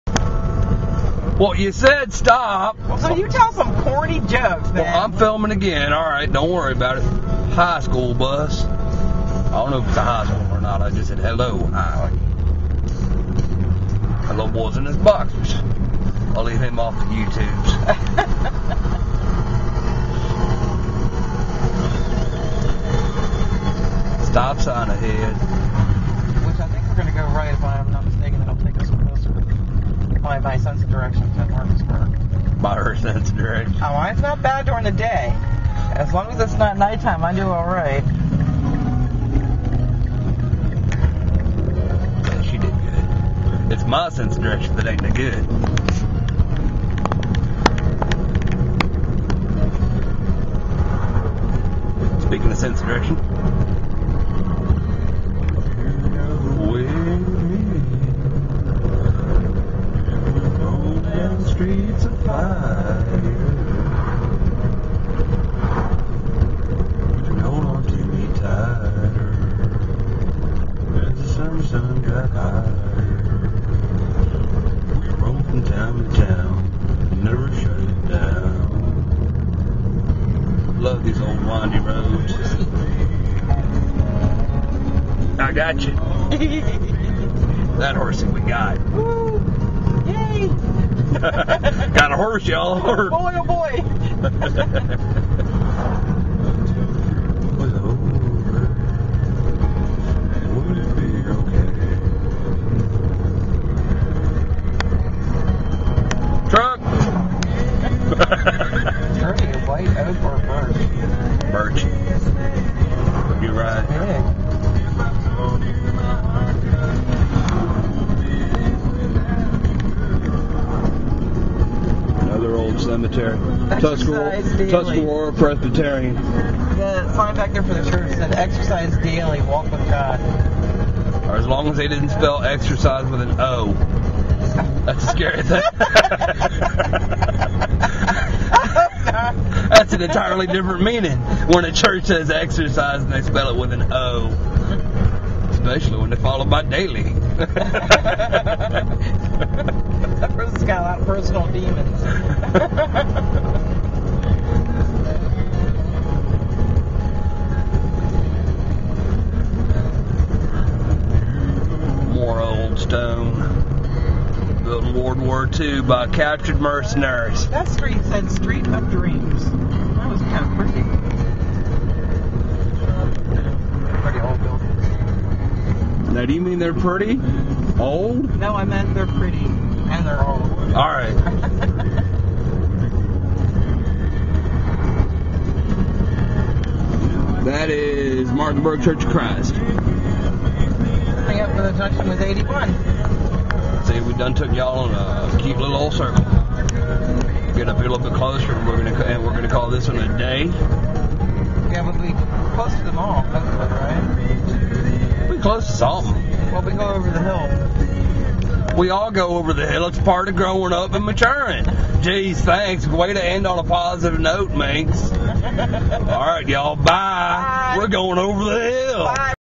Martinsburg, the Long Way pt 4 of 4
takin' a left offa 81 onto 51.. then comin up on 45 ridin' thru Gerrardstown WV .. then a couple purtty roads, passin' orchards, and trailers .. yee haw y'all